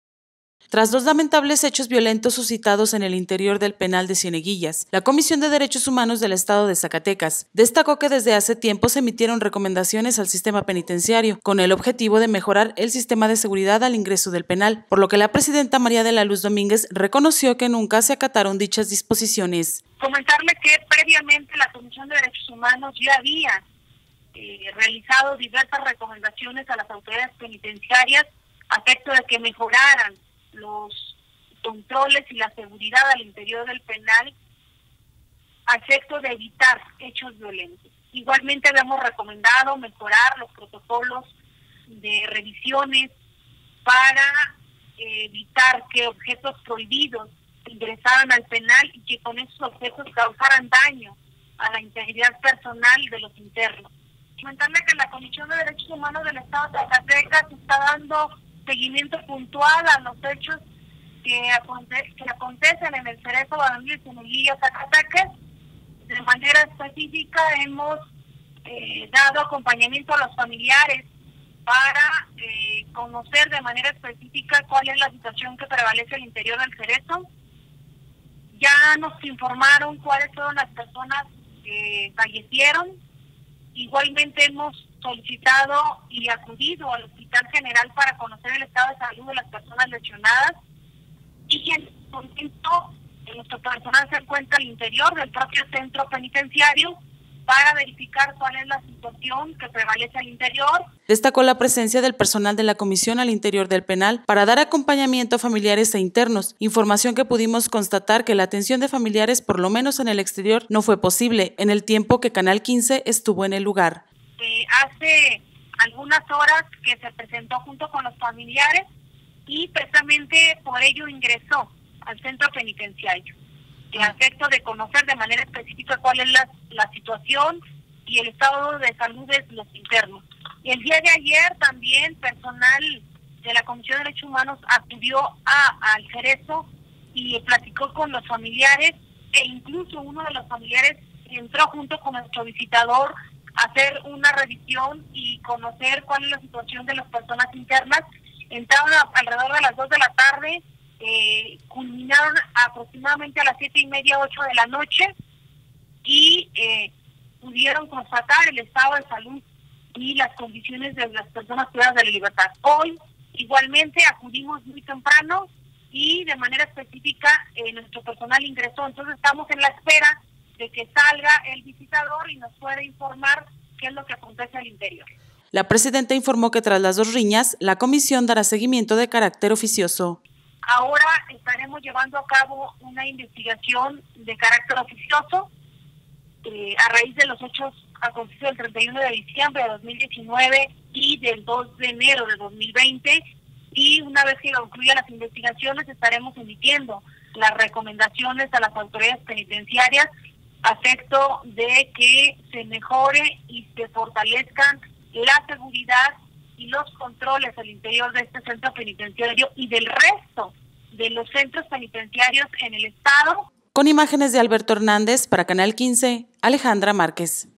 Cerereso recibió recomendaciones de la CDHEZ
Cerereso recibió recomendaciones de la CDHEZ que no fueron acatadas.
Dara seguimiento de carácter oficioso al caso.